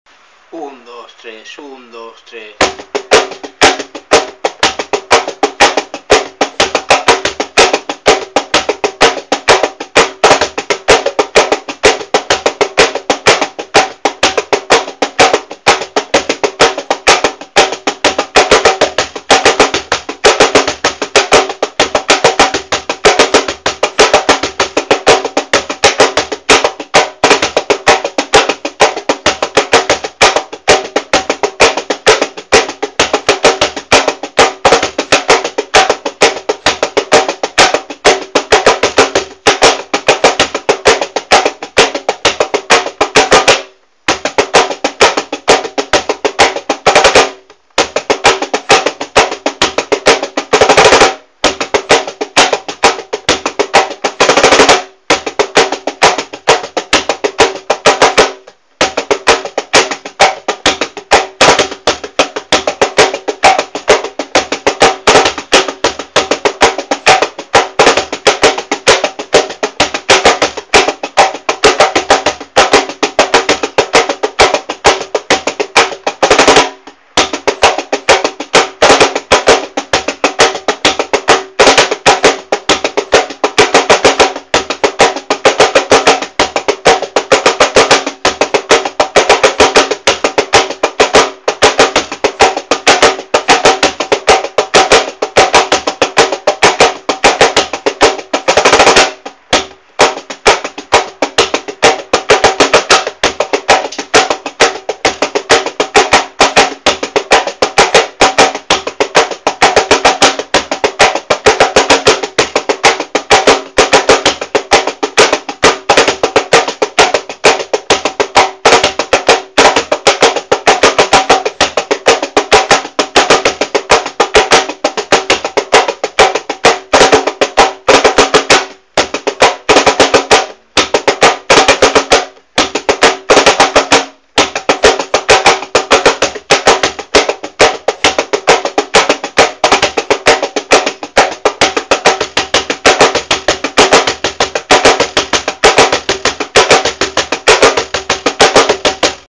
Ejercicios de tanguillos para cajón flamenco
Patrones y clases de cajón flamenco para nivel medio-alto.
Recursos y ejercicios de tanguillos para cajón. Visita o para ver más vídeos clasificados por secciones.